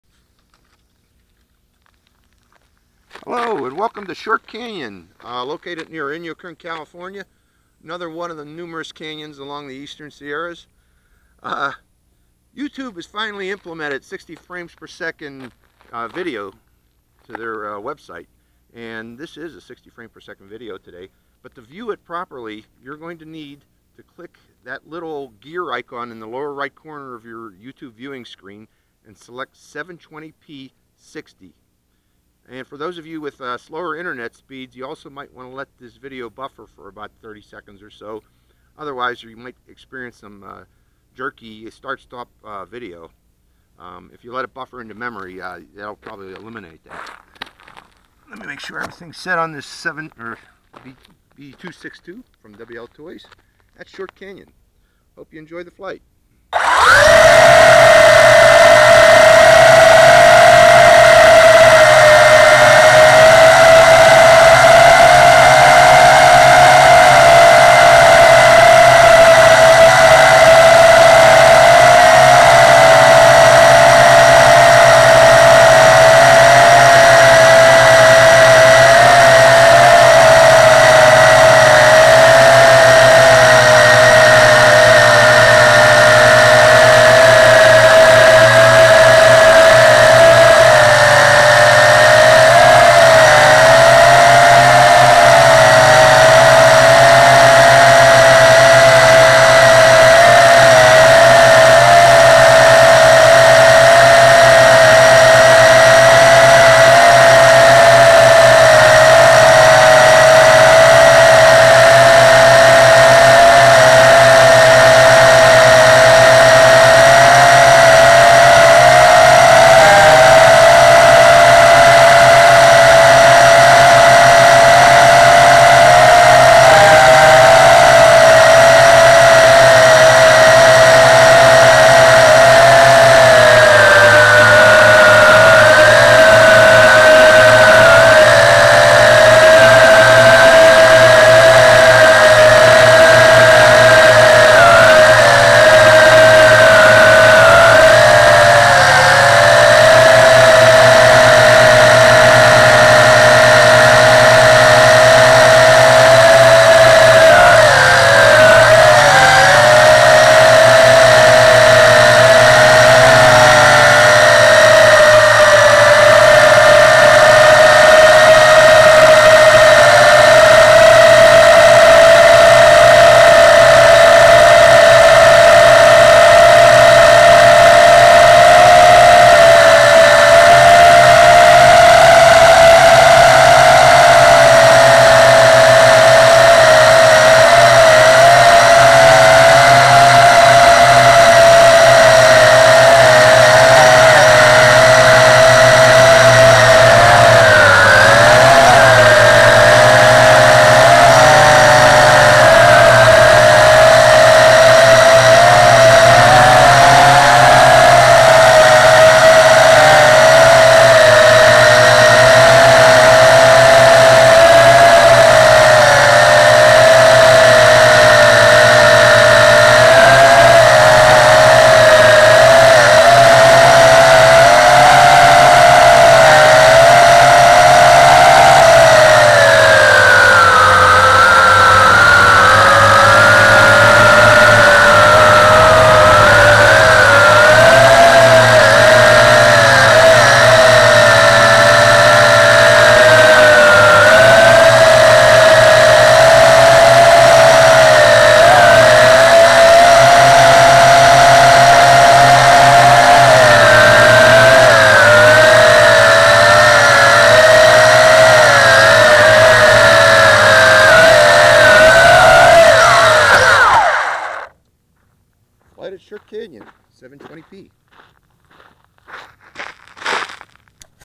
WLToys V262 Quadcopter Drone at 720p60
YouTube has finally implemented 60 fps video!  Quadcopter Drone $90 ($80 for US customers, click on US warehouse to get price ) & Camera $68. 1080p aerial video from a Mobius camera attached to a WLToys V262 quadcopter.

Compare the cost to that of a DJI Phantom and GoPro. If you're a beginner, I highly recommend that you start here before you considering purchasing, and possibly damaging/losing, higher end equipment. The little Mobius camera definitely does provide very high quality video in a small package.  It's low cost (~$68) makes it perfect for high-risk applications such as aerial video as seen here. The WL262 quadcopter drone (~$80) is relatively easy to fly, and is big enough to carry most newer lightweight cameras. And it flies very well as seen here.

New to quadcopters?  A great source for what you'll need to know.